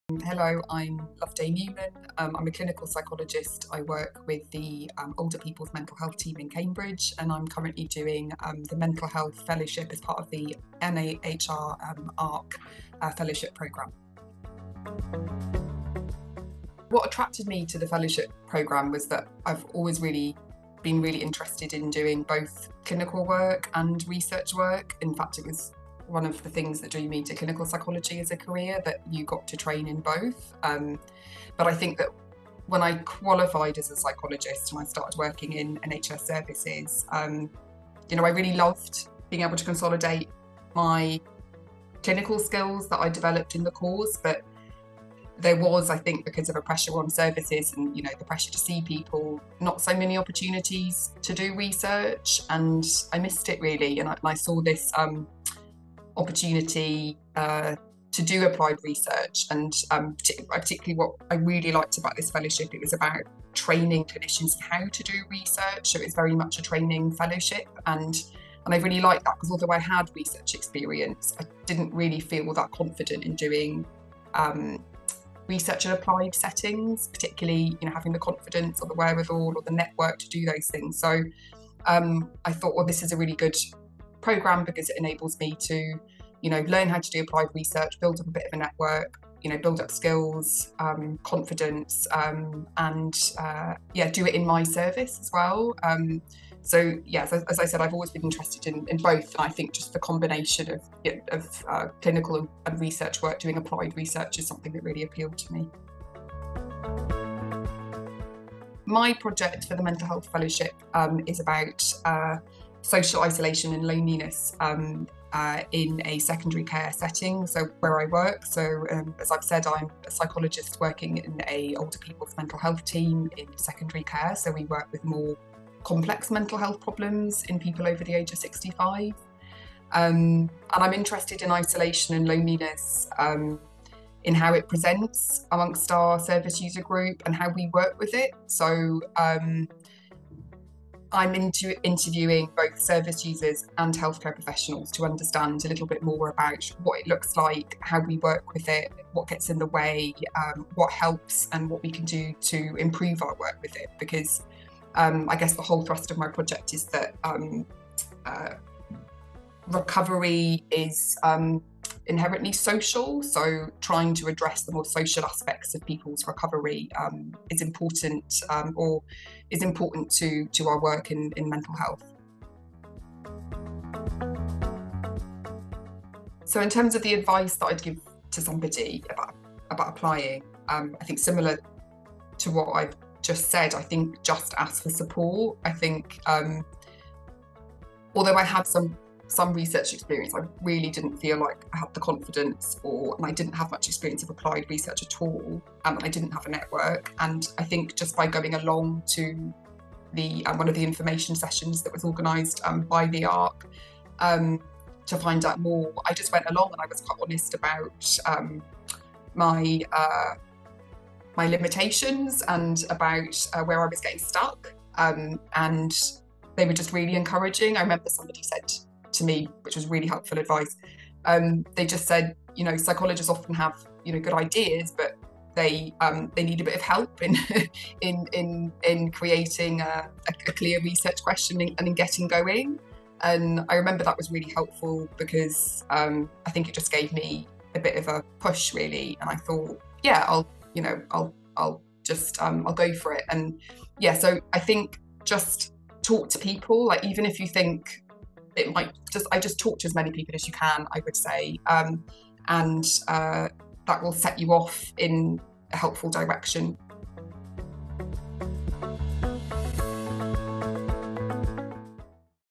Why apply to our ARC East of England Fellowship Programme 2023?
Hear from one of our current ARC East of England Mental Health fellow's, Loveday Newman, who is a clinical psychologist at the Older People's Mental Health team in Cambridge. Loveday discusses why she chose to do a research project with us and what advice she would give someone who is considering applying to our Mental Health Research fellowship programme.

This fellowship is for people based in the East of England who work in an area of mental health, including mental health practitioners, clinicians, NHS managers, health and social care practitioners and those who work in education or third-sector settings

The successful fellows will pursue an 18-month supervised project in an area of mental health. Fellows will spend 12-months on a mental health research project, attend bespoke educational seminars, followed by 6-months working on a mental health implementation project. This fellowship is a collaboration with the Eastern Academic Health Science Network who will be delivering the implementation project.

For more information about how to apply to our fellowship programme: